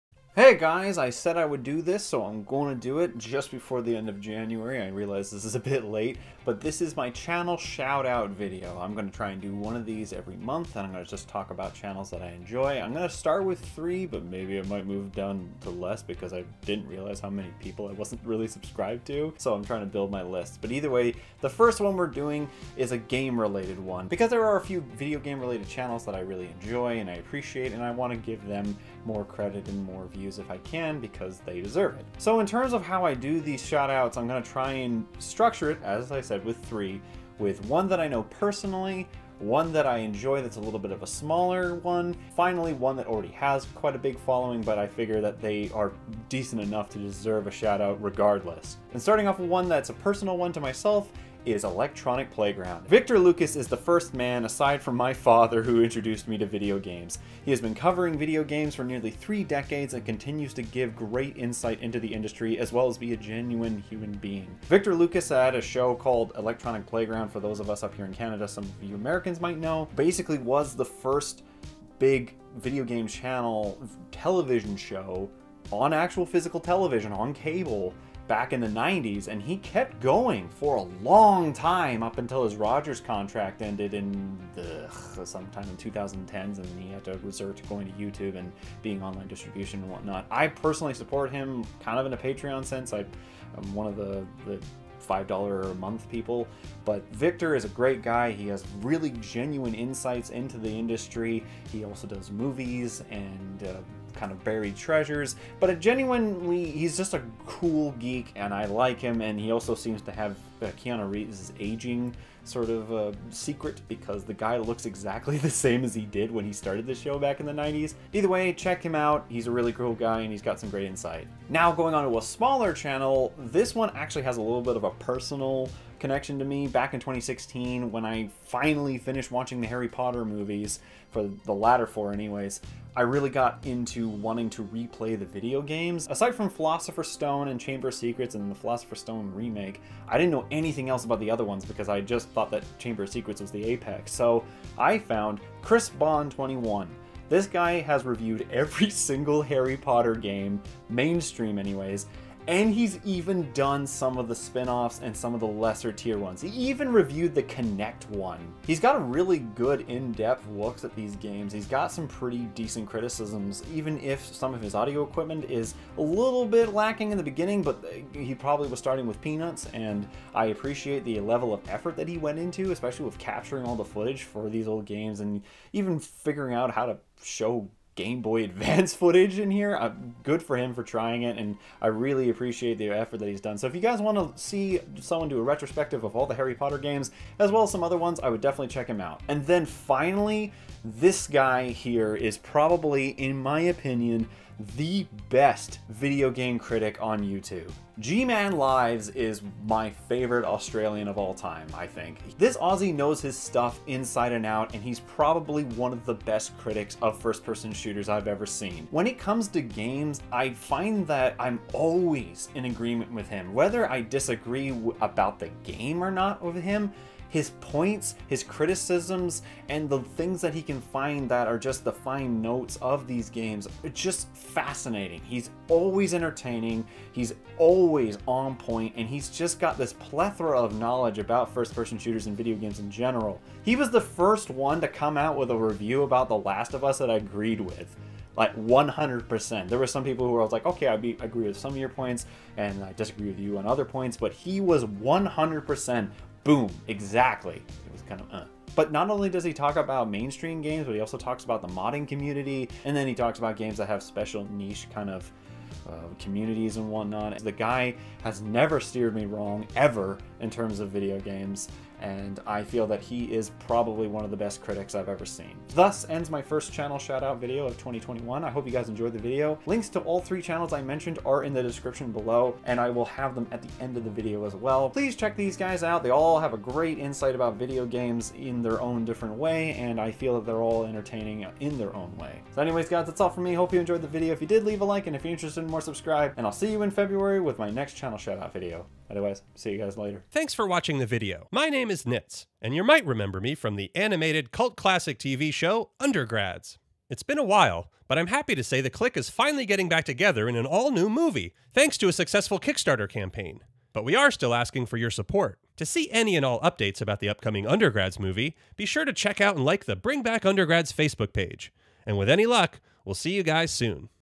January 2021 Channel Shoutouts - Videogame Critics
My first shoutout is about a few video game critics I genuinely enjoy and appreciate their insight and views on video games.

So I'm slipping this in just before the end of the month, here is my first in hopefully a consistent schedule of channel shout outs!

CHANNEL LINKS HERE!